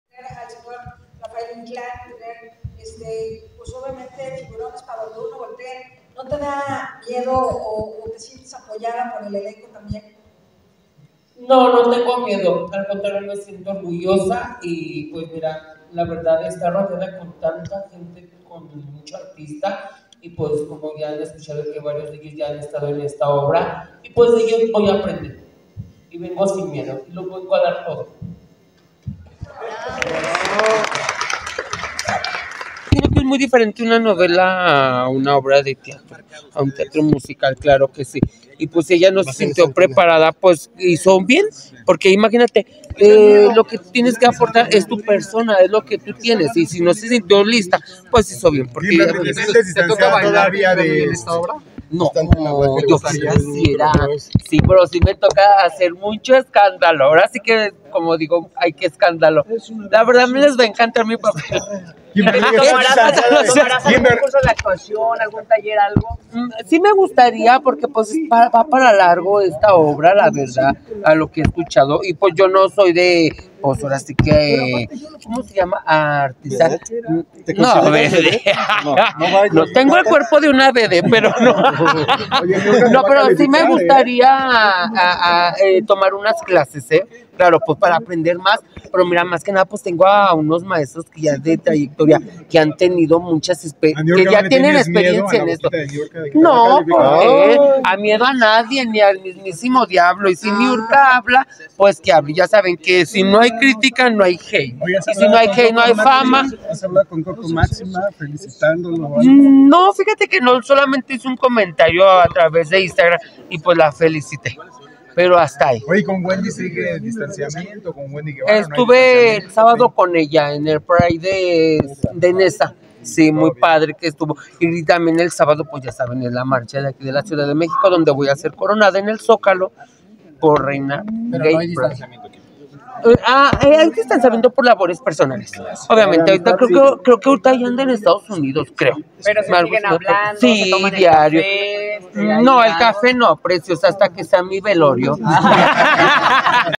Kimberly sin miedo a las críticas de Niurka, en su debut en Perfume de Gardenia!!⚠️
Kimberly la más preciosa sin miedo a las críticas de Niurka, al contrario las espera con ansias, esto por su debut en Perfume de Gardenia.